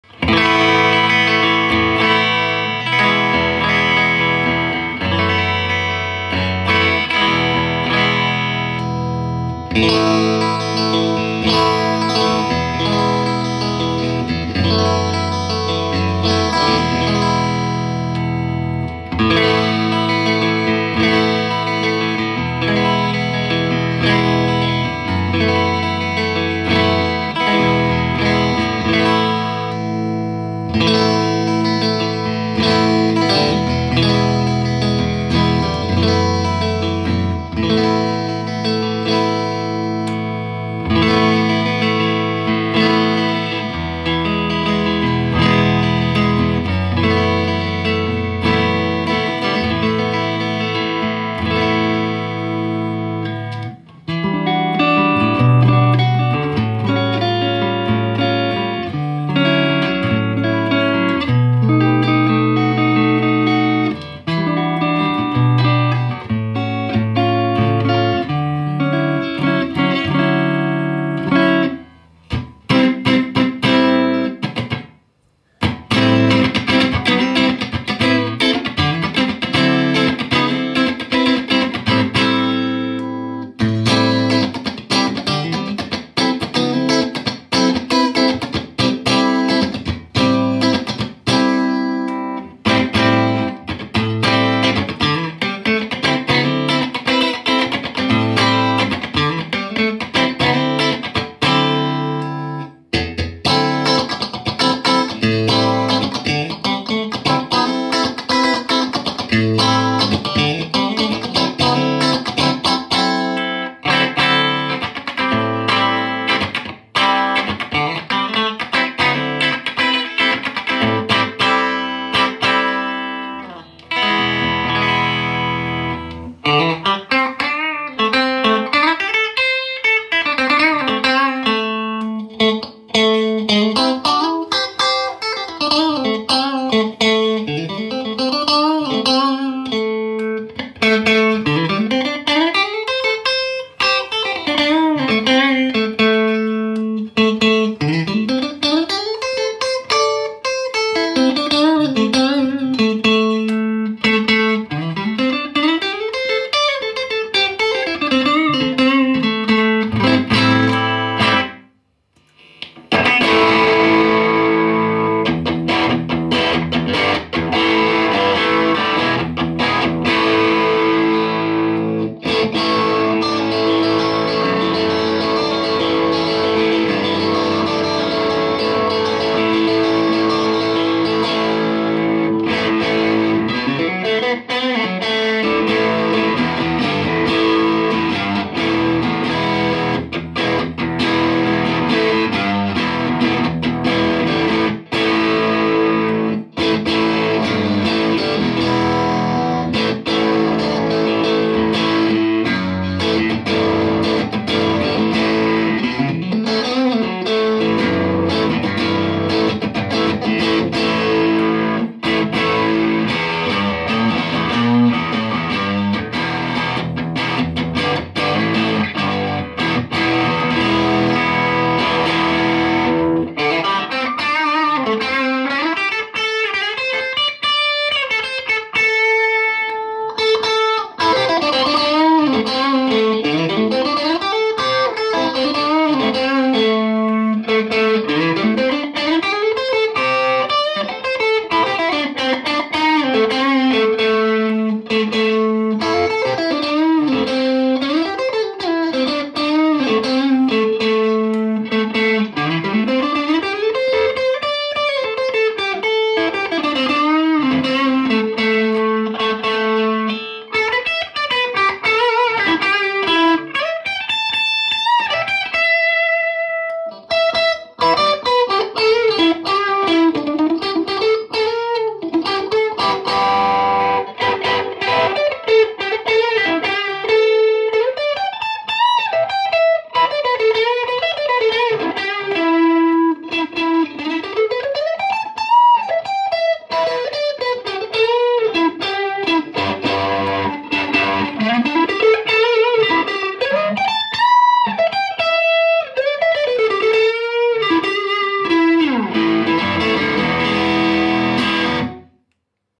Tsubasa Guitar Workshop LUCY ALD/R 3TS-Aged【商品紹介@Guitar Planet】
プロミュージシャンが使用する楽器の制作などを手掛けるビルダー
田中千秋氏によるプロジェクト”Tsubasa”のストラトキャスター・スタイルギター

※弊店にて調整・セットアップされた商品のサウンドサンプルです。